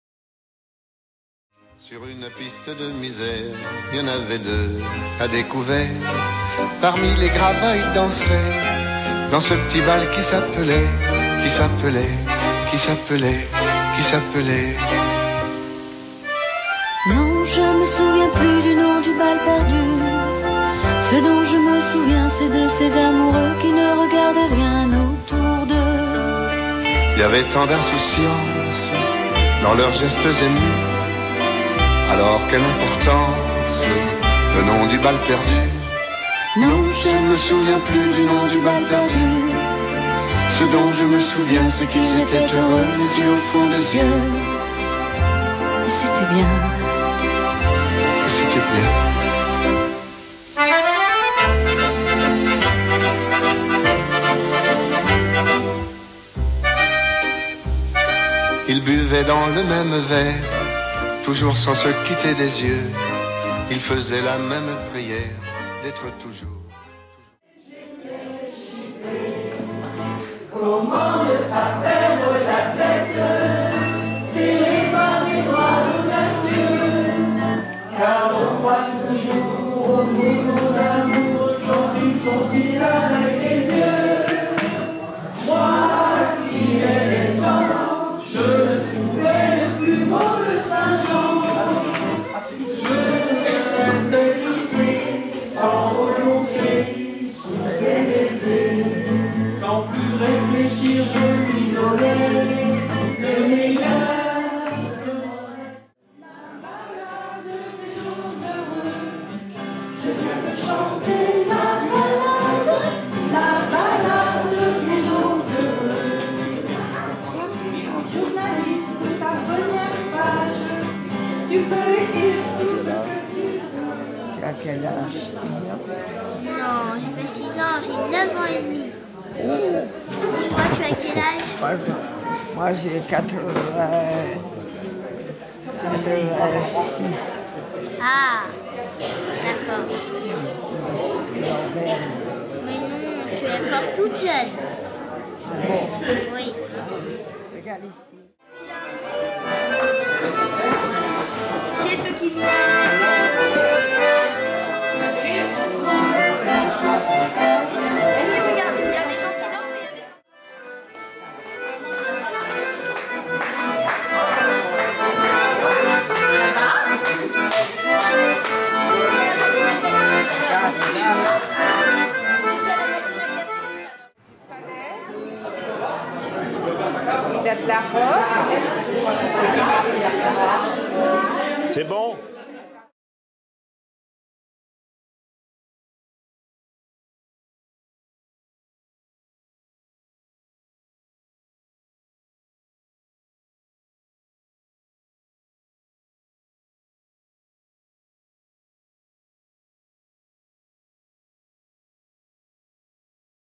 EPHAD août 2017
Un jour pas comme les autres pour Mémé entourée de résidents et de ses proches (enfants, petites enfants et arrières petits-enfants) à l'initiative des animateurs de l'EHPAD.